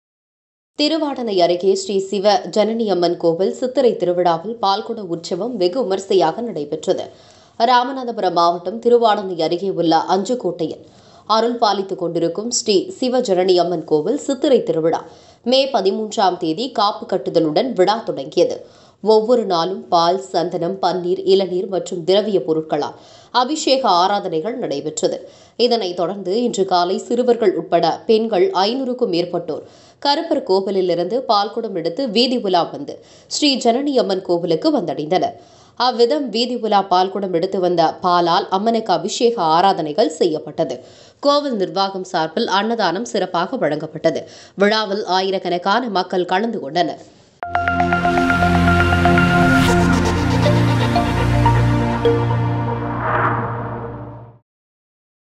ஸ்ரீ சிவ ஜனனியம்மன் கோவில் சித்திரை திருவிழாவை முன்னிட்டு பால்குட உற்சவம் வெகு விமர்சையாக நடைபெற்றது
ராமநாதபுரம் மாவட்டம் திருவாடானை அருகே உள்ள அஞ்சுகோட்டையில் அருள்பாலித்து கொண்டிருக்கும் ஸ்ரீ சிவ ஜனனி அம்மன் கோவில் சித்திரை திருவிழா மே 13ம் தேதி  காப்பு கட்டுதலுடன் விழா தொடங்கியது. ஒவ்வொரு நாளும் பால், சந்தனம், பன்னீர், இளநீர், மற்றும் திரவிய பொருக்களால்  அபிஷேக ஆராதனைகள் நடைபெற்றது. இதனை தொடர்ந்து இன்று காலை சிறுவர்கள் உட்பட பெண்கள் 500க்கும் மேற்பட்டோர் கருப்பர் கோவிலில் இருந்து பால்குடம் எடுத்து வீதி உலா வந்து ஸ்ரீ ஜனனியம்மன் கோவிலுக்கு வந்தடைந்தனர். அவ்விதம் வீதி உலா பால்குடம் எடுத்து வந்த பாலால் அம்மனுக்கு அபிஷேக ஆராதனைகள் செய்யபட்டது.